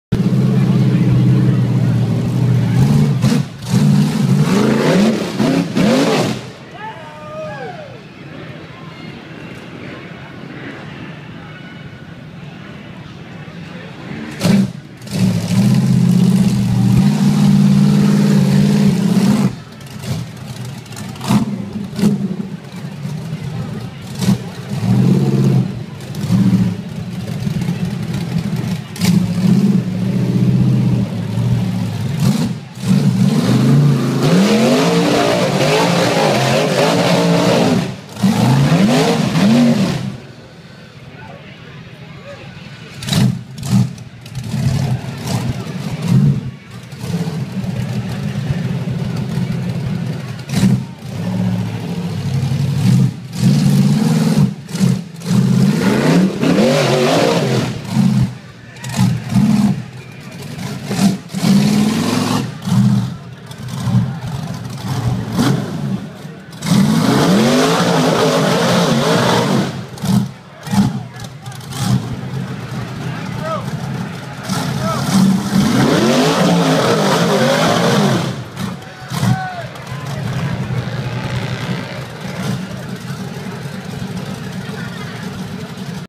Plow boy let's his buddy Bobby Tanner hammer down
Plow boy buggy breaks a front axle shaft on dirty turtle's "getya sum" beat down!